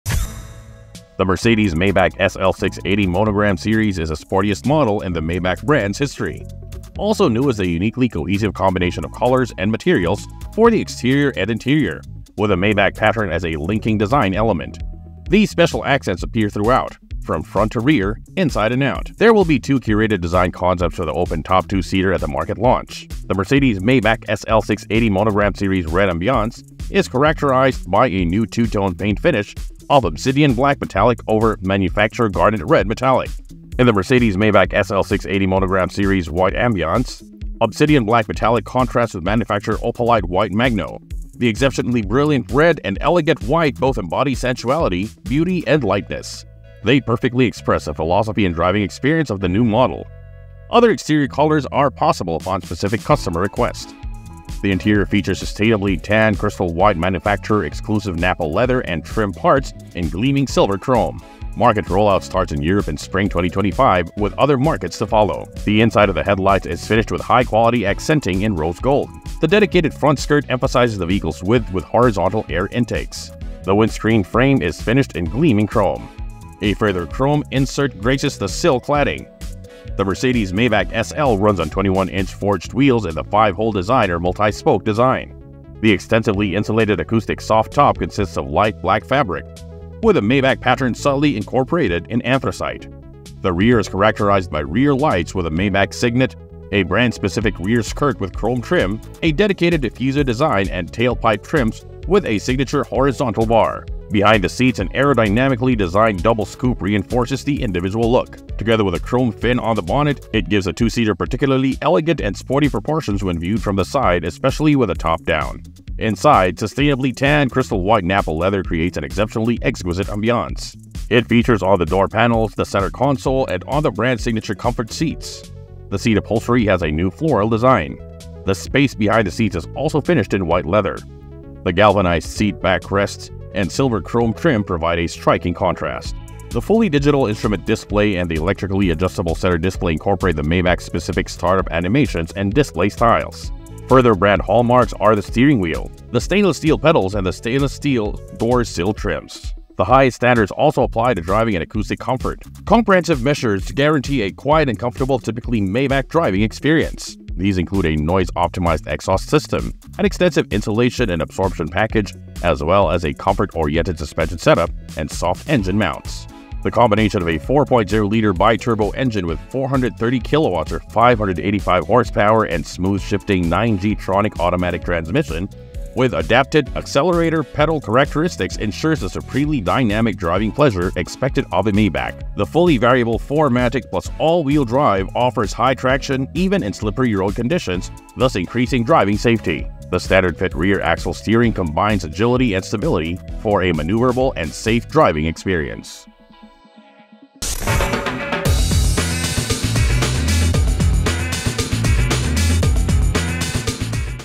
2024 Mercedes-Benz Maybach SL Monogram Series | Autos of the World
The Mercedes-Maybach SL 680 Monogram Series is the sportiest model in the Maybach brand's history. Also new is the uniquely cohesive combination of colors and materials for the exterior and interior, with the Maybach pattern as a linking design element.